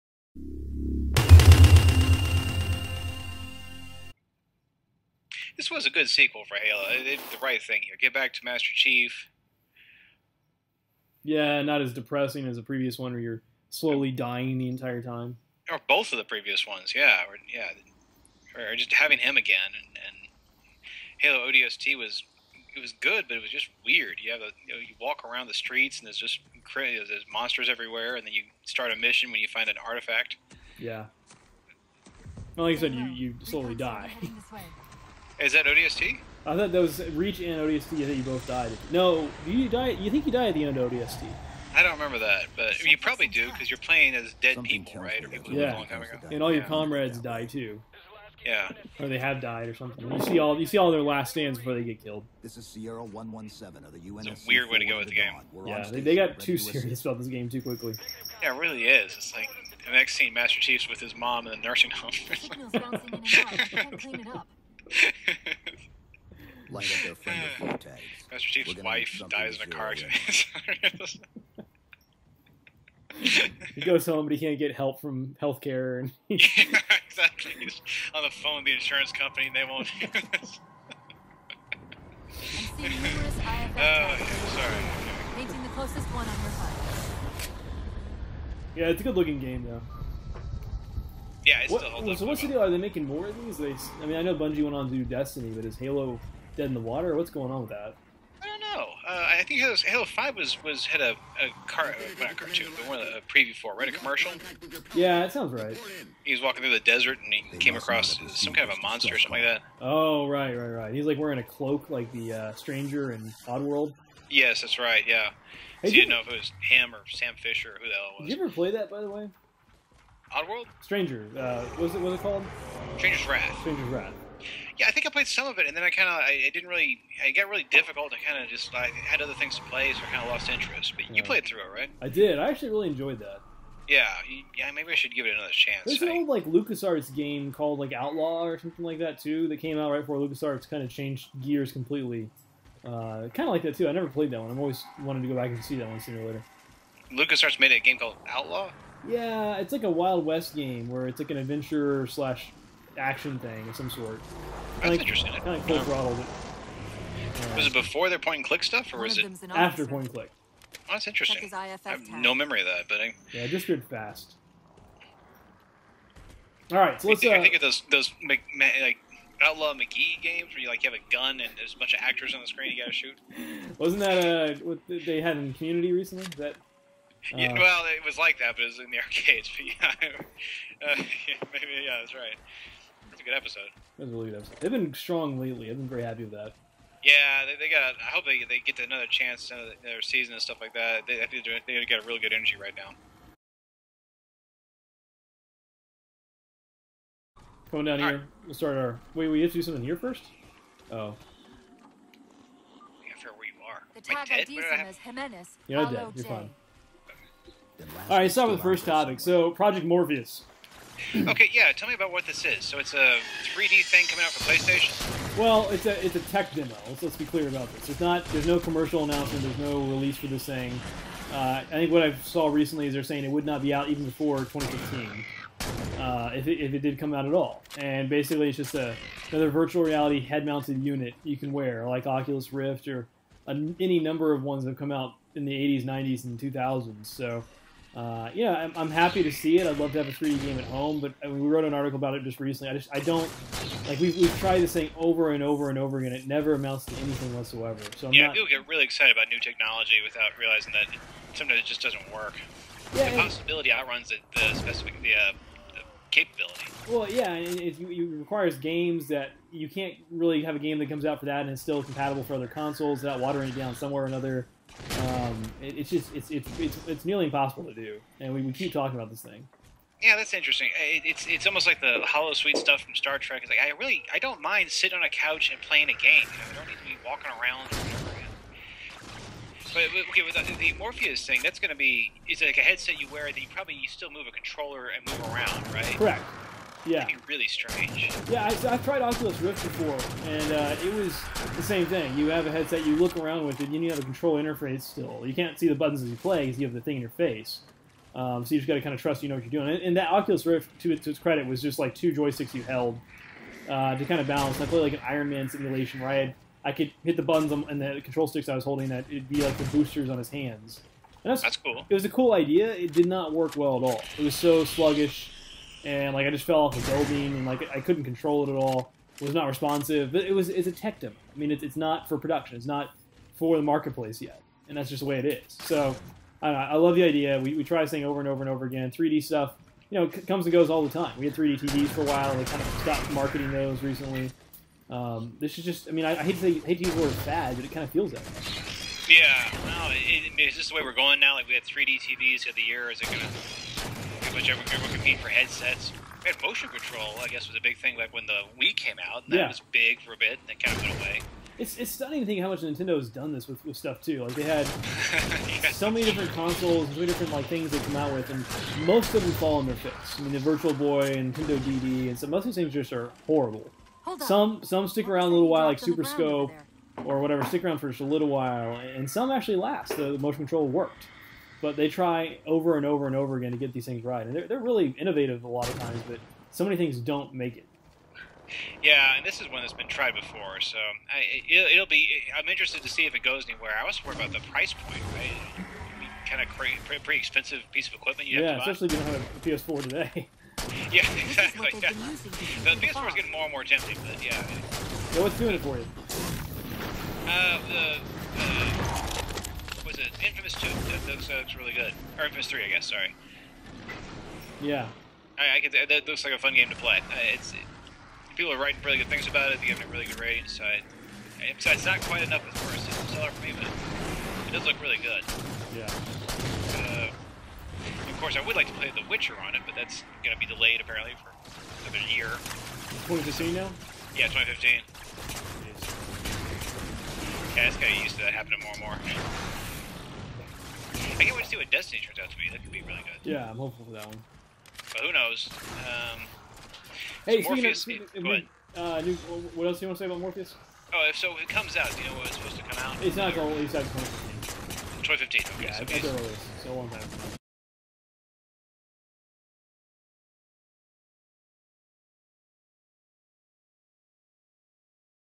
Play Friday - This week in news for all things geek (3 -21 - 14)
We tried to bring it back with one topic this week but then lost our audio file after a brief glitch on one side.  Oh well, we'll be back next week with a full topic list.  This week, we just talk more about 3d games and how ultimately lackluster they are.  Enjoy!